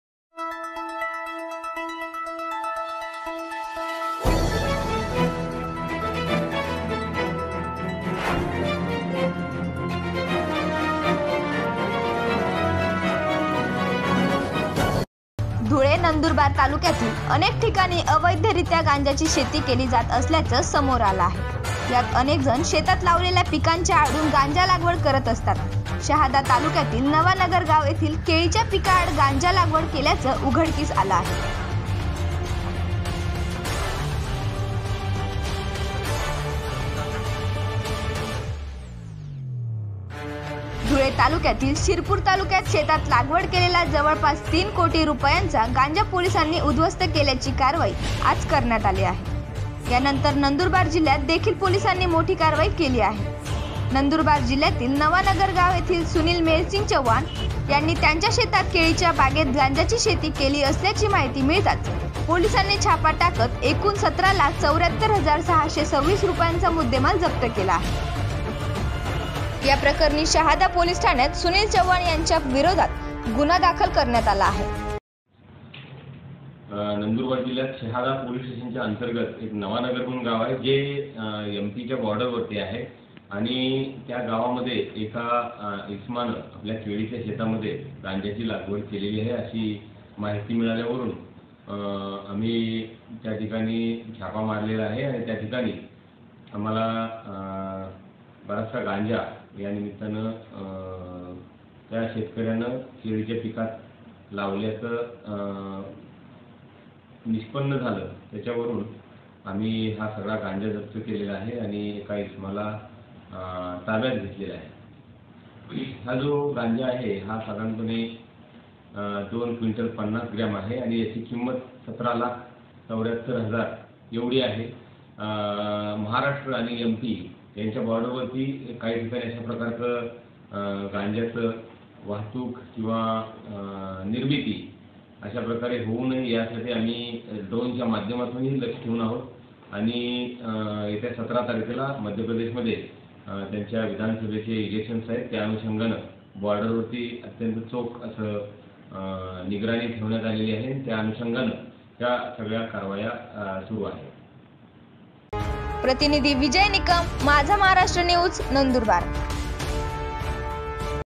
केळीच्या बागेत पोलिसांची धाड. 17 लाखाचा गांजा केला जप्त.
धुळे, नंदुरबार जिल्ह्यातील अनेक ठिकाणी अवैधरित्या गांजाची शेती केली जात असल्याचे समोर आल आहे. यात अनेकजण शेतात लावलेल्या पिकांच्या आडून गांजा लागवड करत असतात. शहादा तालुक्यातील नवानगर गाव येथेही केळीच्या पिका आड गांजा लागवड केल्याच उघडकीस आलं आहे...